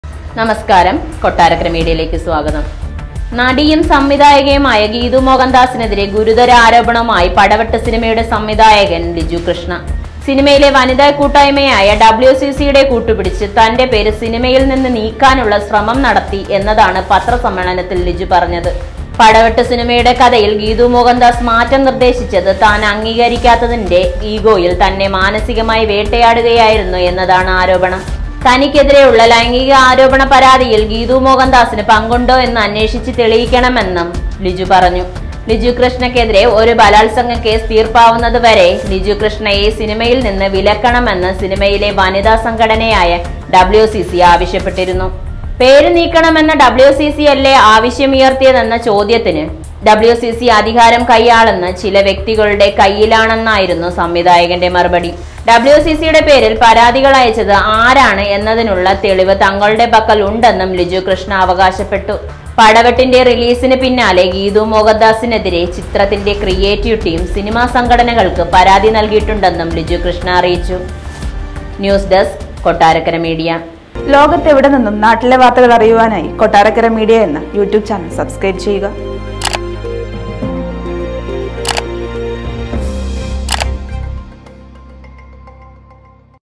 Film Industry News | ലിജുവും ഗീതുവുമായി പടവെട്ട്‌ | Liju Krishna Complaint Against Geethu Mohandas
Looking for film industry news? Check out this story about Liju Krishna, the padavettu film director, filing a complaint against Geethu Mohandas in a press meet. We'll be discussing this complaint and what it means for the film industry in this video.  This news story is sure to interest film fans everywhere!

കൊട്ടാരക്കരക്കാരുടെ സ്വന്തം വാർത്താ ചാനൽ ആണ് കൊട്ടാരക്കര മീഡിയ